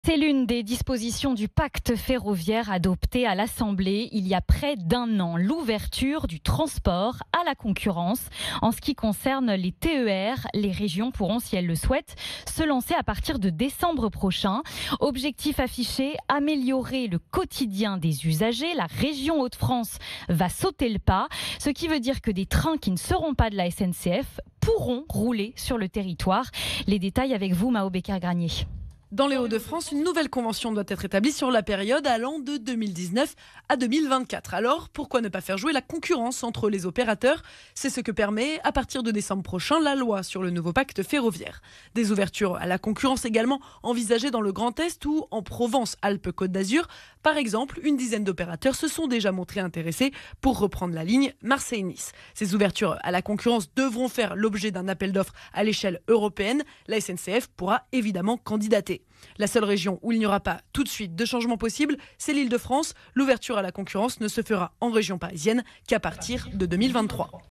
Les petites lignes ferroviaires pourront bientôt être ouvertes à la concurrence
La région Hauts de France a décidé d'ouvrir les lignes TER à la concurrence pour faire face aux problèmes rencontrés sur ses lignes ferroviaires. C'est l'une des dispositions du pacte ferroviaire adopté à l'Assemblée il y a près d'un an.